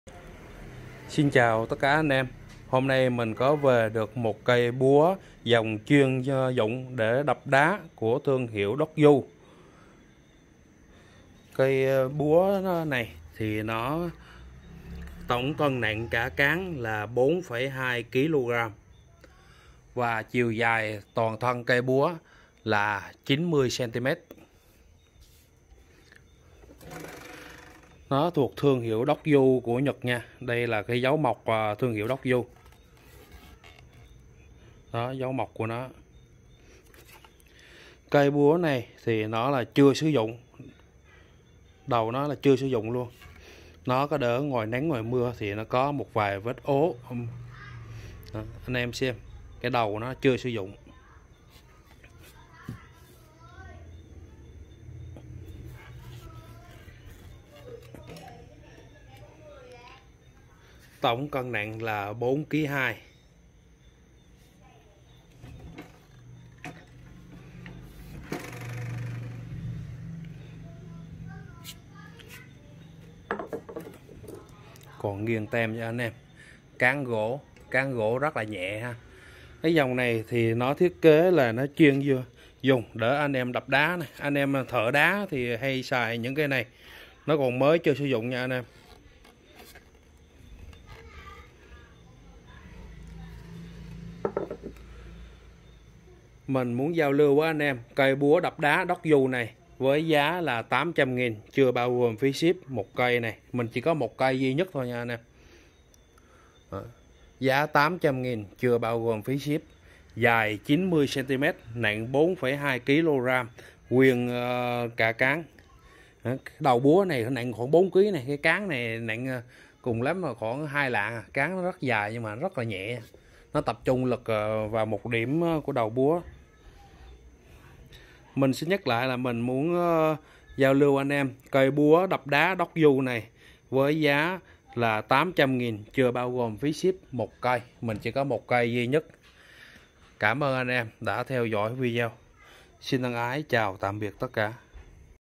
Búa đập đá dogyu. 4.2kg dài 90cm giá 800k + ship lh mr 0355734853
Búa chuyên dụng đập đá của dogyu made in japan. Hàng chưa sử dụng. Tổng  cân nặng 4.2kg dài 90cm. Chỉ 1 cây duy nhất giá 800k chưa bao gồm phí vận chuyển nhé anh em. Ship cod toàn quốc lh mr đại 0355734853